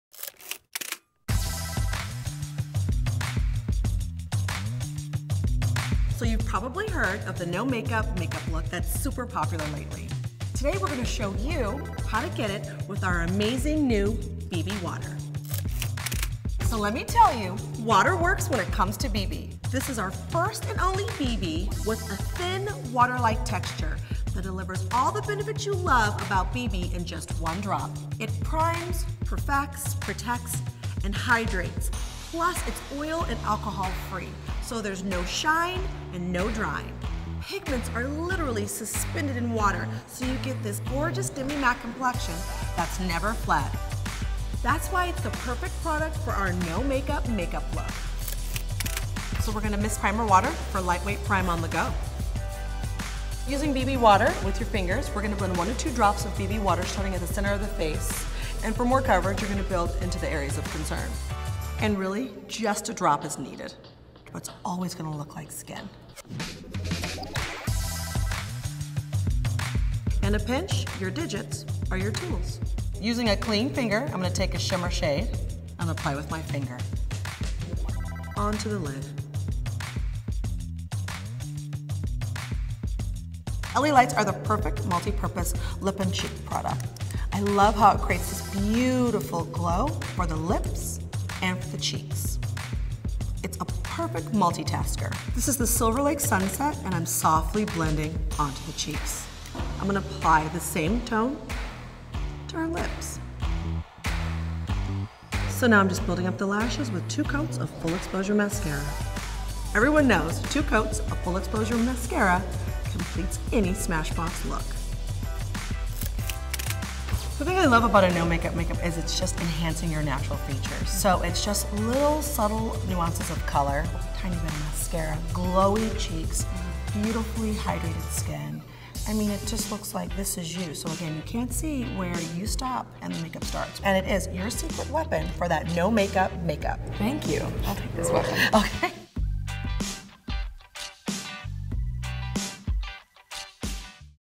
Smashbox Camera Ready BB Water
WHAT IS IT? Your skin needs a drink. Meet our first and only BB with a water-like texture that delivers multiple benefits in just one drop. Why get wet? Our built-in primer water technology means a liquid silk-consistency - it literally glides onto your face, blends easily and moves with your skin. Because it's alcohol-free, there is no drying, cracking or caking during wear. The ultra-hydrating, oil-free formula also creates a sheer, radiant finish that makes you really love your skin in every conceivable light. Here come the compliments.

WHY IT'S UNIQUE.
Simply put, we have no other BB foundation with a thin, water-like veil. What makes it a technological feat is our breakthrough system that suspends pigments in H2O. The big pay-off: a BB that hydrates your skin throughout the day and a demi-matte, but never flat complexion. Just a few drops create a remarkably natural-looking canvas that still camouflages imperfections. Just build for more coverage. Plus, our exclusive Photoset Polymers - taken from medical grade liquid bandage technology - make it last from A.M. to afterhours.

BRAGGING RIGHTS
- PRIMES: Blurs out imperfections like fine lines, pores and blemishes
- PROTECTS: SPF 30 helps shield skin from environmental damage
- PERFECTS: Sheer-to-medium coverage comes easy with its buildable, liquid-silk consistency. Plus, 8 forgiving shades means one for every skin tone. Guaranteed.
- HYDRATES: A revolutionary formula infused with elements of Primer Water quenches skin
- OIL & ALCOHOL FREE: Expect a gorgeous sheen. No shine.

ONE-TWO PUNCH. Want that all natural off-duty model look? Done. After applying BB Water, add a healthy flush or subtle, sun-kissed sheen to your flawless skin with our new L.A. Lights Blendable Lip & Cheek Color.

THE PHOTO FACTOR. "It all starts with skin in the studio. A sheer finish on the face reflects natural light better and highlights contours like cheekbones." - Davis Factor, Founder and Photographer

ARTIST HOW-TO: KEEP IT SHEER. "Remember, it's all about perfecting - not masking your skin. You only need to apply foundation on places that need coverage." - Lori Taylor Davis, Global Pro Lead Artist